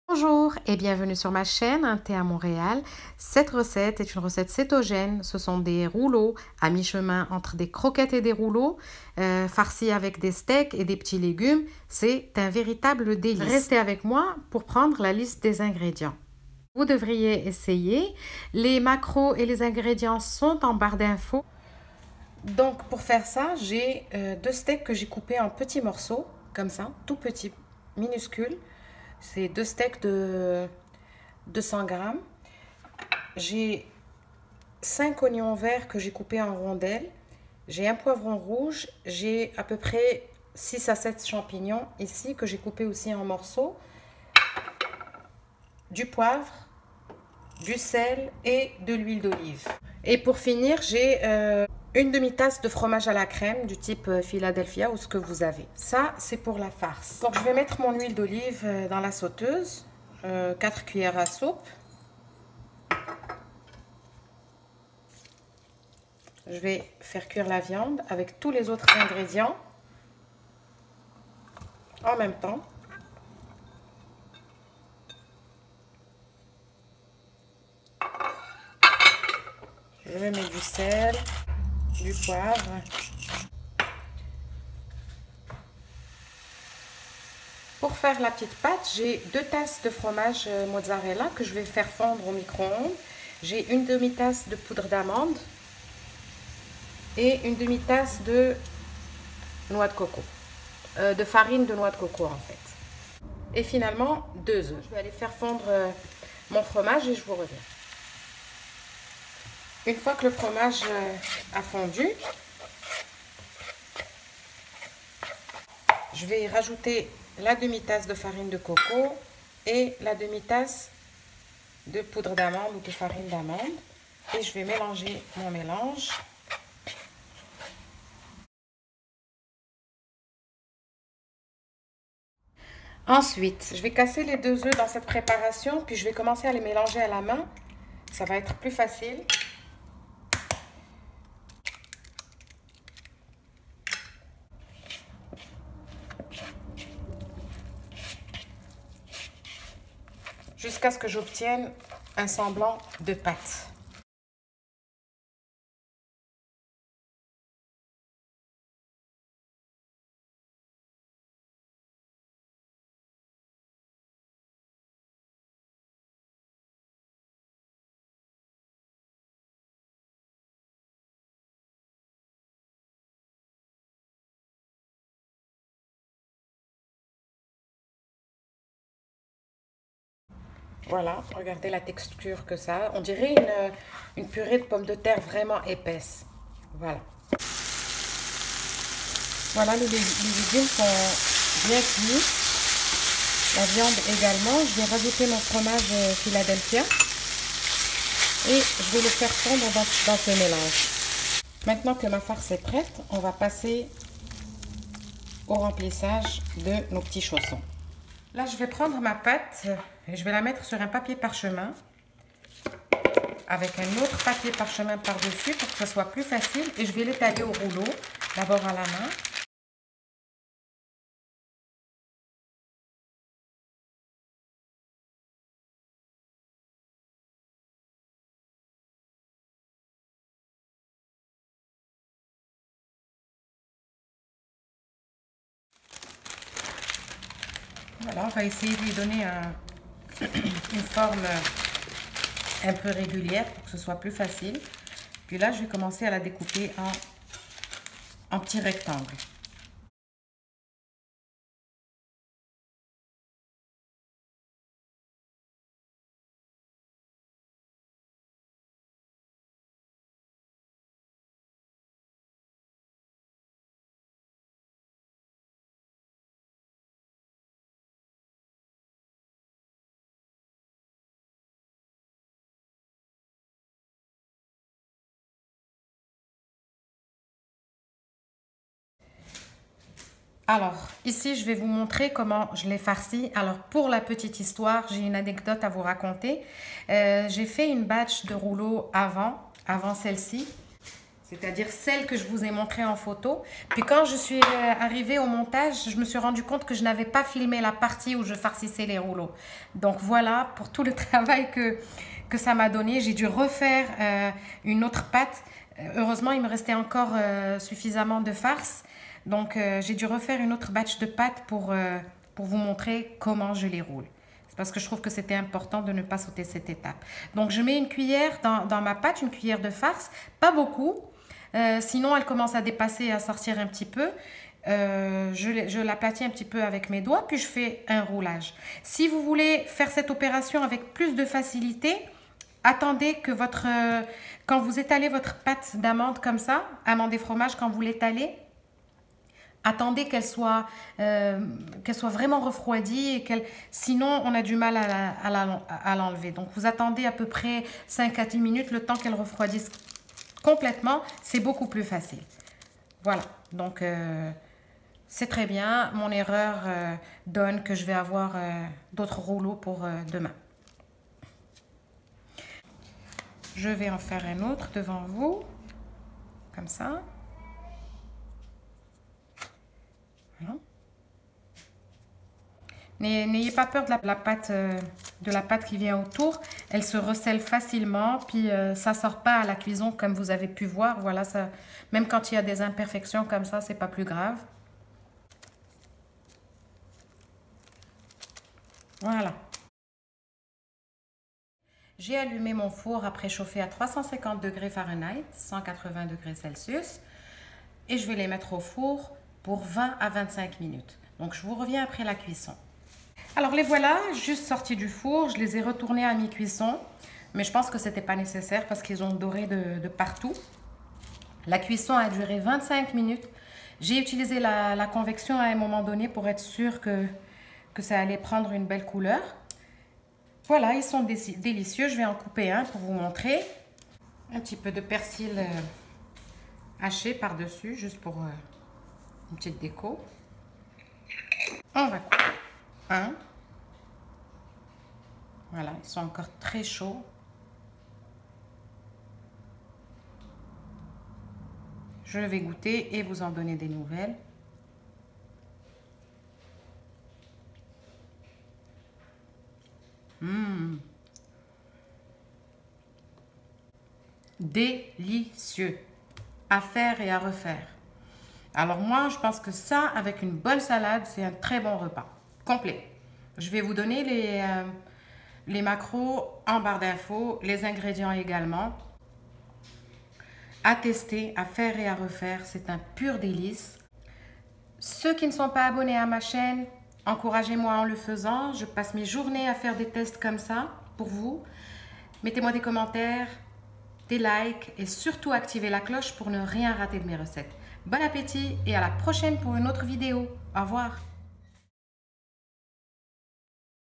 Rouleaux Ou Croquettes Cétogènes
INGRÉDIENTS

Pour la pâte :

2 tasses de mozzarella râpée ( 400g)
1/2 tasse de farine d’amandes
1/2 tasse de farine de noix de coco
2 œufs

Pour la farce :

200g de steak
5 à 6 oignons verts
un poivron rouge
6 champignons de Paris
½ tasse de fromage crémeux

MACROS:

Pour un rouleau
220 calories
14g de protéines
6g de glucides
20de gras